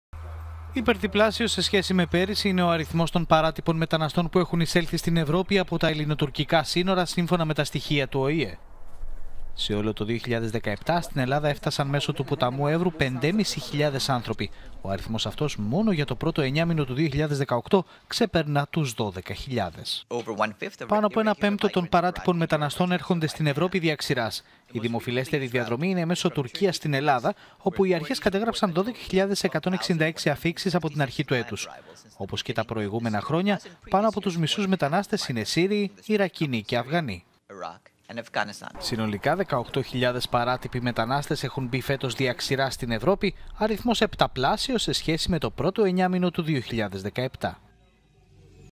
Ελλάδα 2018.Ανενόχλητοι εισέρχονται κάθε λογής αλλοδαποί στη χώρα
Δυστυχώς οι μετανάστες δεν...λιάζονται απλά και έπειτα εξαφανίζονται μυστηριωδώς από τους ελληνικούς δρόμους.Αντιθέτως πληθαίνουν ολοένα και περισσότερο, με τις αφίξεις αλλοδαπών από τον Έβρο να λαμβάνουν διαστάσεις λαίλαπας.

Ενδεικτικό είναι πως ο Έβρος τις τελευταίες μέρες χαρακτηρίστηκε ως το δημοφιλέστερο πέρασμα των μεταναστών στην Ευρώπη.Αν μη τι άλλο η φήμη δεν προηγείται αυτή τη φορά των γεγονότων, καθώς χρόνο με τον χρόνο οι μεταναστευτικές ροές υπερπολλαπλασιάζονται στην περιοχή του Έβρου, χαρακτηρίζοντας τα ελληνικά σύνορα ως ένα «ξέφραγο αμπέλι» πολλών δεκάδων χιλιομέτρων.

Ιδιαίτερα δε στους καλοκαιρινούς μήνες όπου οι στάθμη των υδάτων σε πολλές περιοχές του Έβρου είναι χαμηλή, Τούρκοι και Έλληνες διακινητές περνάνε ακόμη πιο εύκολα τους μετανάστες (στην πλειοψηφία τους Σύριοι) στο ελληνικό έδαφος.

Ακόμη και ο ΟΗΕ έχει σημάνει τον κώδωνα του κινδύνου για την κατάσταση που επικρατεί στα ελληνοτουρκικά σύνορα του Έβρου, με την ελλιπή φύλαξη αλλά και τις αφίξεις παράτυπων μεταναστών να λαμβάνει διεθνείς διαστάσεις:

«Πάνω από ένα πέμπτο των παράτυπων μεταναστών έρχονται στην Ευρώπη δια ξηράς. Η πλέον δημοφιλής διαδρομή είναι μέσω Τουρκίας στην Ελλάδα.

Οι αρχές κατέγραψαν 12.166 αφίξεις από την αρχή του έτους. Όπως και τα προηγούμενα χρόνια, πάνω από τους μισούς μετανάστες είναι Σύριοι, Ιρακινοί και Αφγανοί», επεσήμανε ο αναπληρωτής εκπρόσωπος του γενικού γραμματέα του ΟΗΕ Φαρχάν Χακ.

Αξίζει δε να σημειωθεί πως συγκριτικά με την αντίστοιχη περσινή περίοδο, φέτος ο αριθμός των παράτυπων μεταναστών που εισήλθαν στην Ευρώπη μέσω ξηράς ανήλθε συνολικά τους 18.000, αυξημένος κατά 7 φορές!Η κατάσταση στον Έβρο τείνει να επιδεινωθεί και απεικονίζει το τεράστιο πρόβλημα της φύλαξης των ελληνοτουρκικών συνόρων.

Δεν θα ήταν υπερβολή να ειπωθεί πως η περιοχή του Έβρου έχει μετατραπεί σε «παράδεισο» των διακινητών παράτυπων μεταναστών.

Την δεδομένη κατάσταση την οποία κατά καιρούς εκμεταλλεύονται οι πιο «θερμόαιμοι» Τούρκοι πολιτικοί.

Είναι εκείνοι που απειλούν συνεχώς με «πλημμύρα μεταναστών» την αφύλακτη Ελλάδα, δίνοντας ως παραδείγματα που ήδη τα βλέπουμε να συμβαίνουν στην Βόρεια Ελλάδα.

Όλα αυτά στην Ελλάδα των…ανοικτών συνόρων: